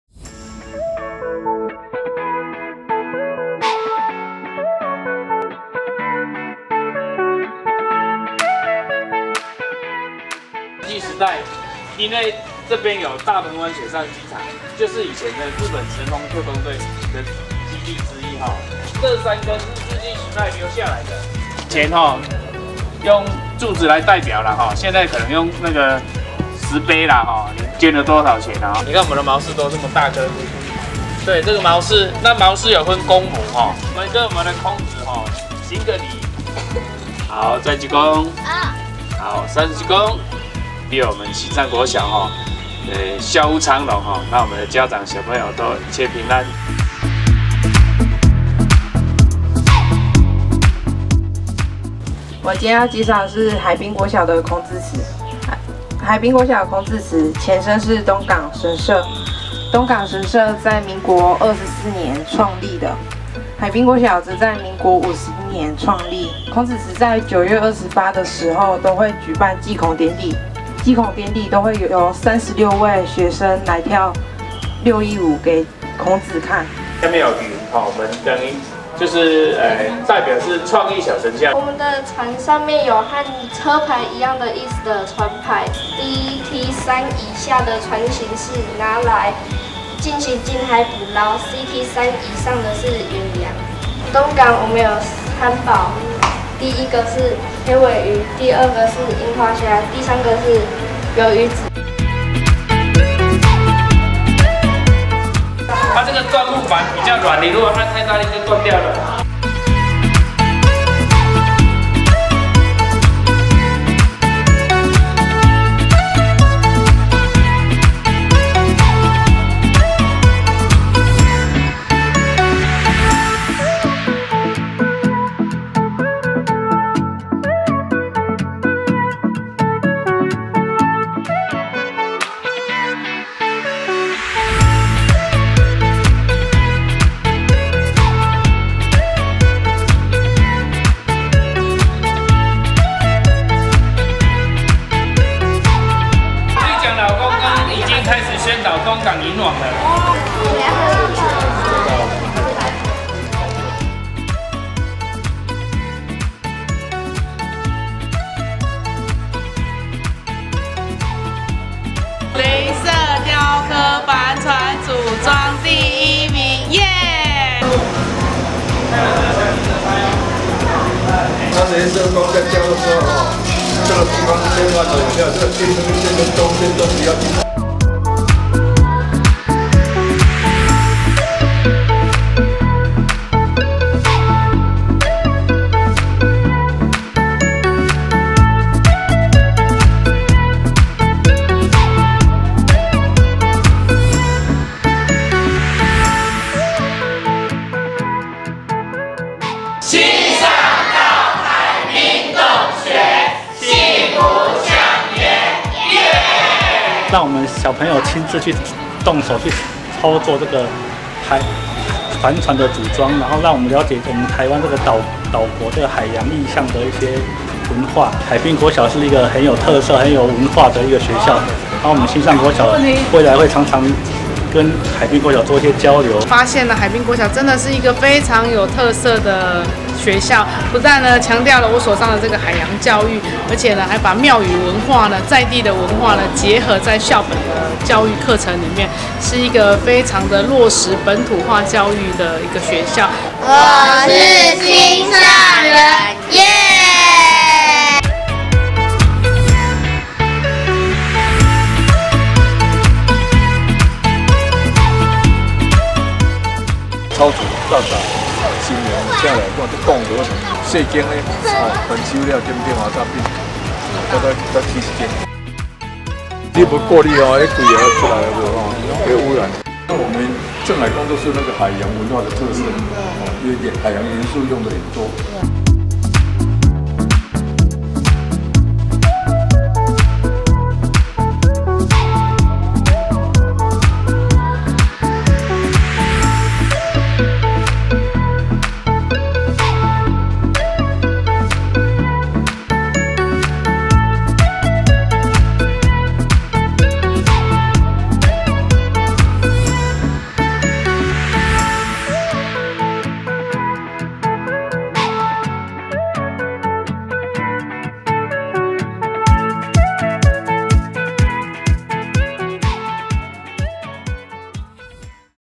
20201111孔子意象雷射雕刻帆船組裝課程及鎮海宮參訪影片
20201111孔子意象雷射雕刻帆船組裝課程及鎮海宮參訪影片
課程主題：孔子意象雷射雕刻帆船組裝課程及鎮海宮參訪
~孔子祠導覽解說-六甲孔儷欣
~東港漁港導覽解說-五甲吳富傑
~孔子意象雷射雕刻帆船組裝課程
~孔子祠祈福卡色鉛筆彩繪課程
~東港鎮海宮實地導覽及參訪
~感謝高雄市新上國小家長會蒞臨共襄盛舉